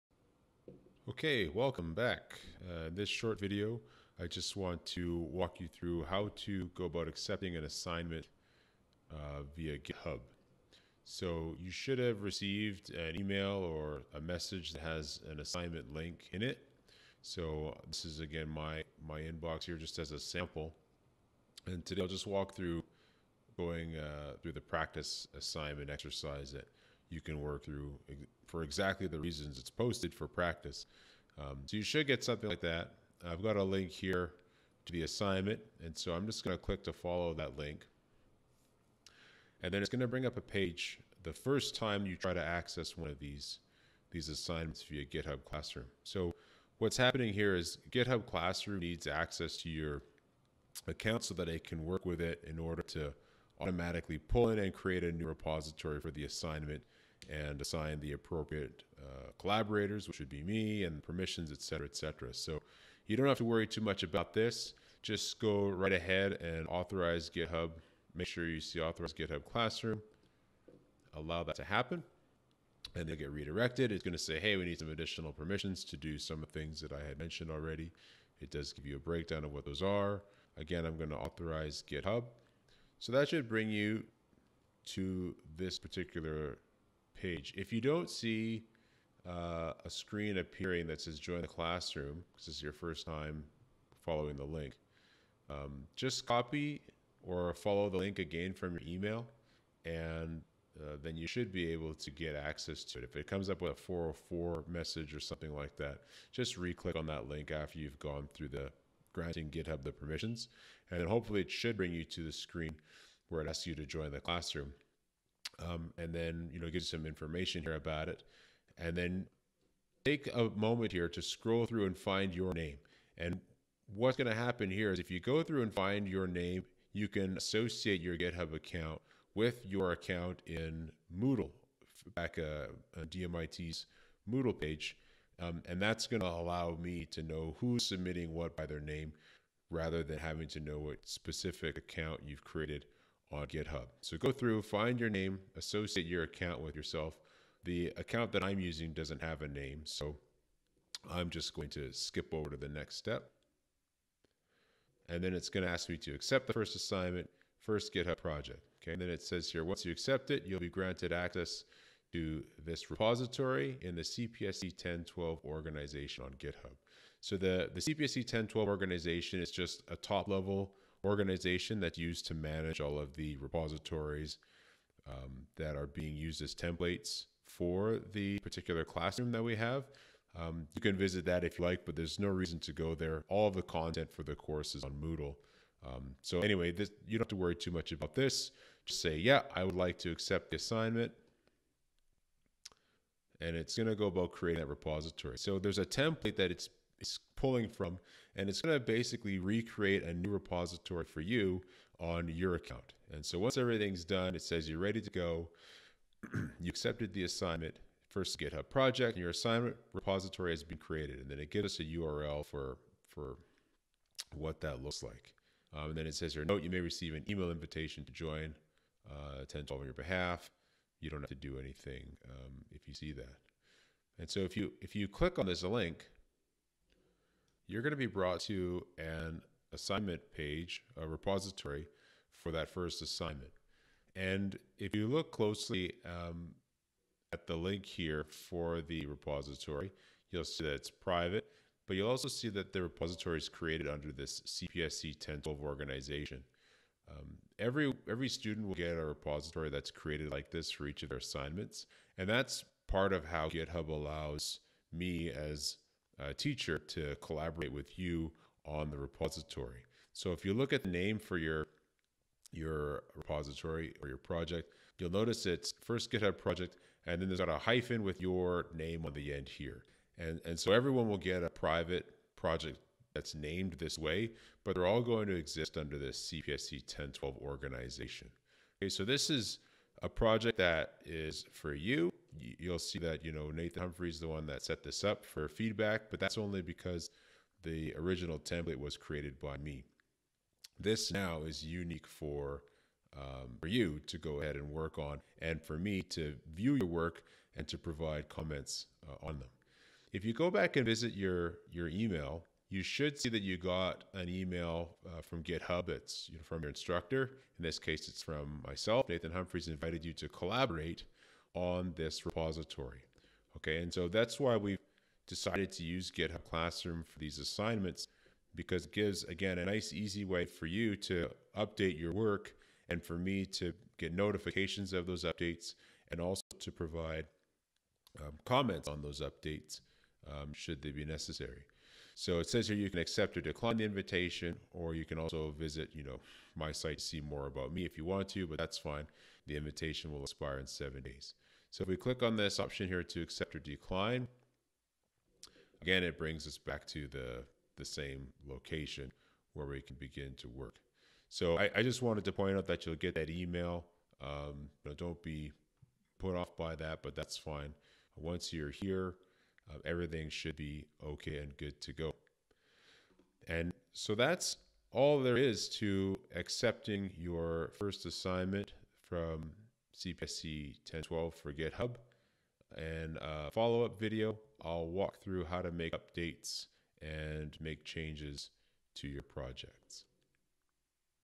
How-To: Accept a GitHub Classroom assignment
A short walkthrough guide showing you how to accept an assignment via GitHub classroom.